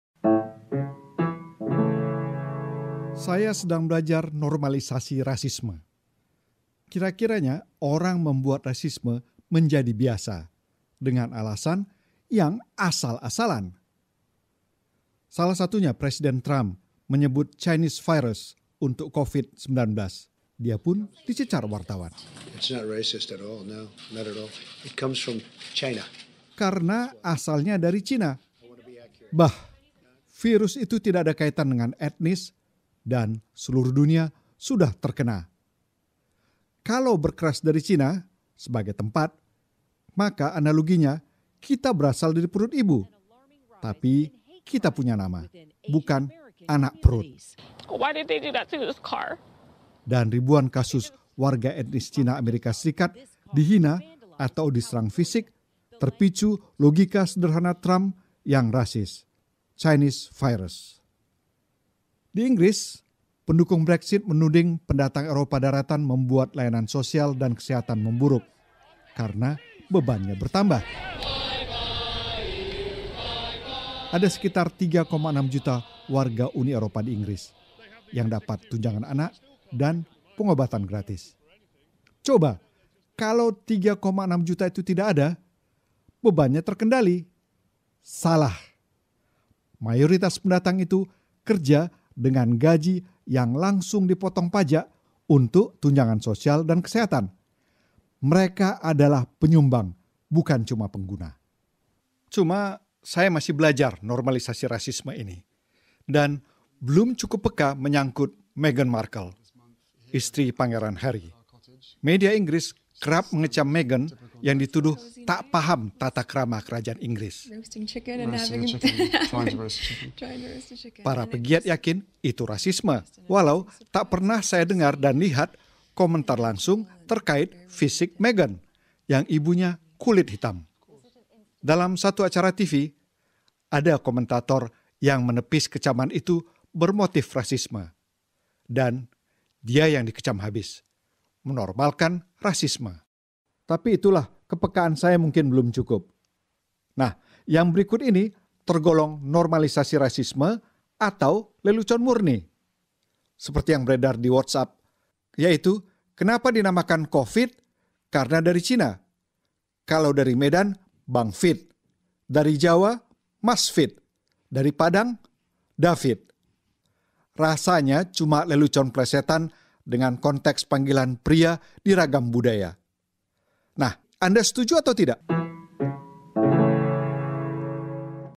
Normalisasi Rasisme - Liston P Siregar
Beberapa orang mencari alasan yang kedengaran masuk akal untuk membuat rasisme sebagai hal biasa. Itu dia: Normalisasi Rasisme.